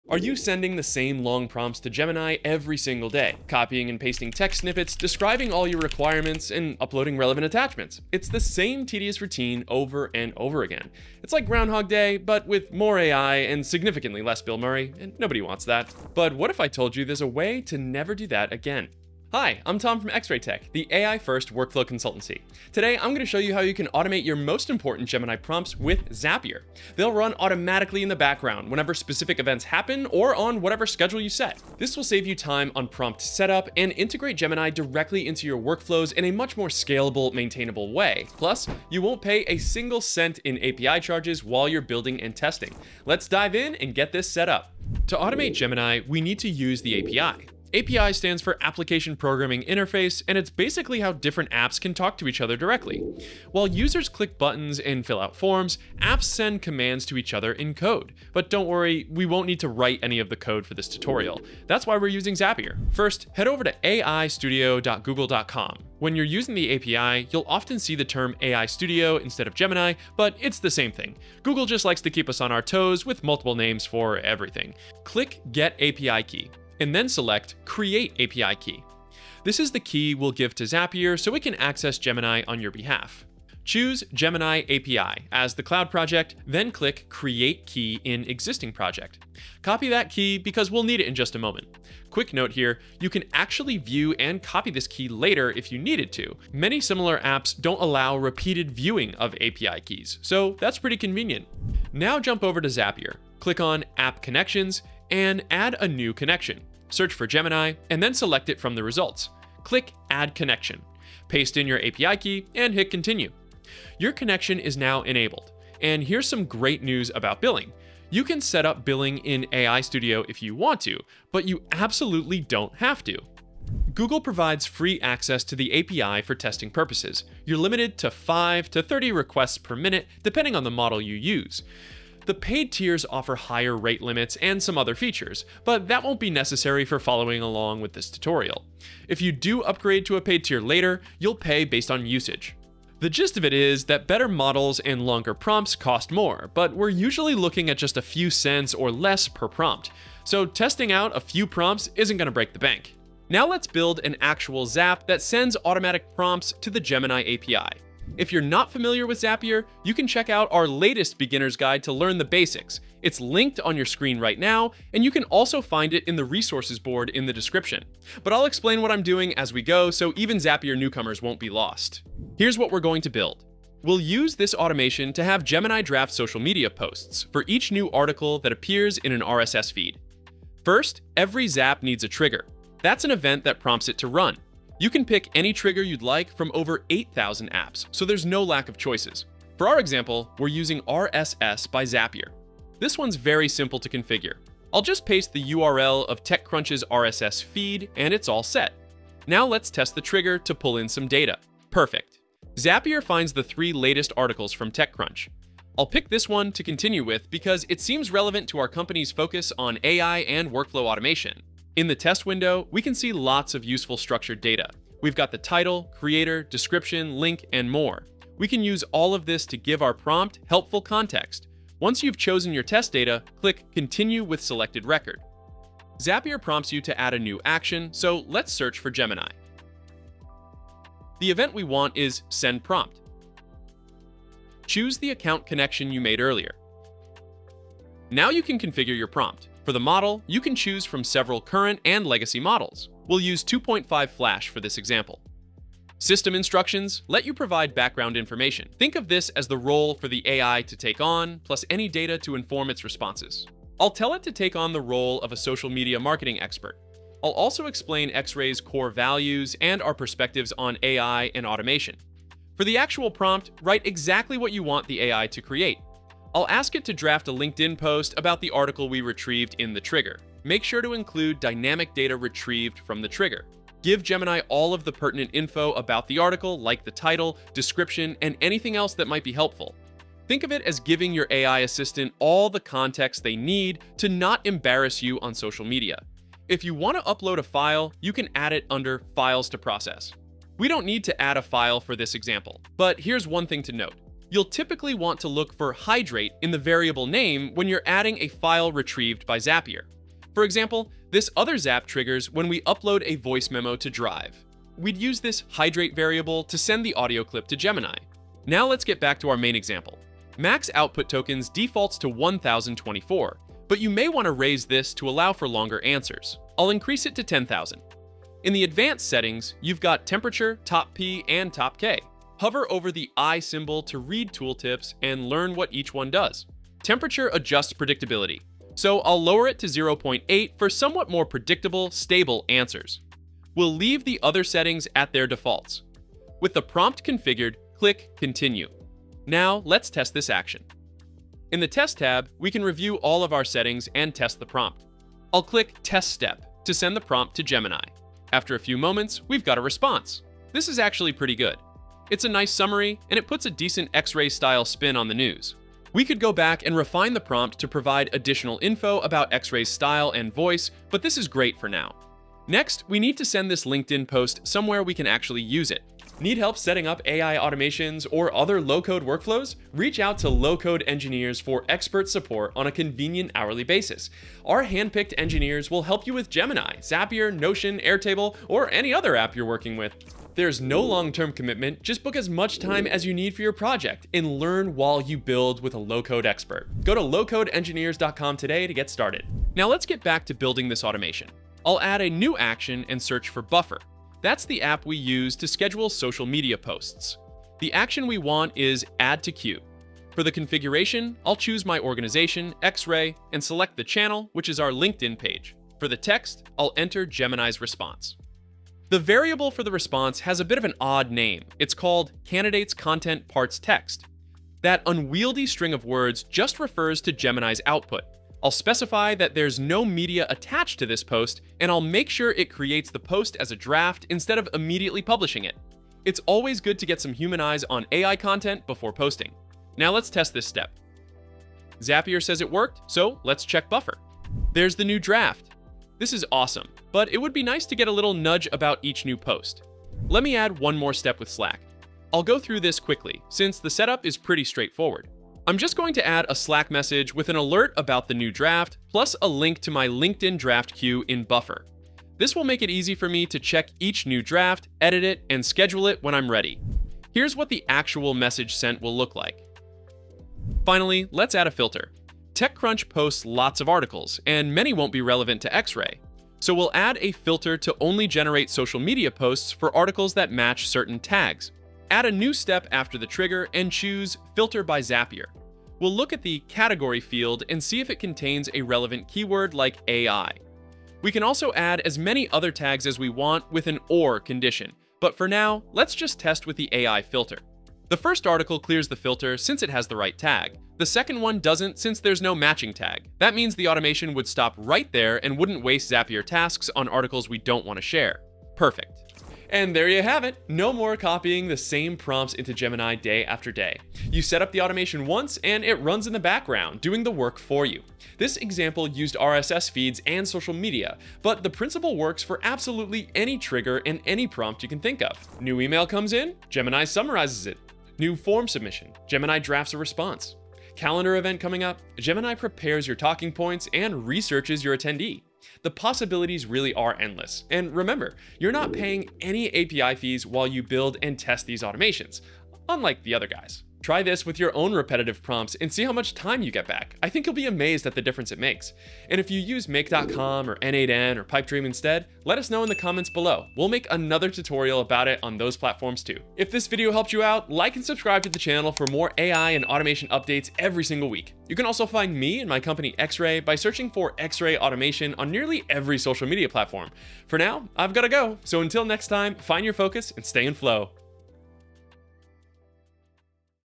Automate Your Gemini Prompts with Zapier
Turn your Gemini AI prompts into consistent, repeatable workflows by automating them with Zapier. I'll show you how to get started in this step-by-step guide.
In our example automation, I'll show you to create an AI-drafted LinkedIn post for every new item in an RSS feed. Then, we'll send the draft to Buffer, and create an alert about it in Slack. Finally, we'll add some smart filtering to ensure our automation only runs for relevant articles.
🤖 Transform your company with AI and automation:
Discover related resources:
00:00 - Intro
00:45 - Connect Gemini API to Zapier
02:38 - Zapier beginner's guide
02:53 - Create a Zap trigger
03:45 - Configure a Gemini prompt
06:05 - Get expert support from LCE
06:31 - Send the AI's output to another app
07:15 - Add a Slack alert
07:40 - Add a filter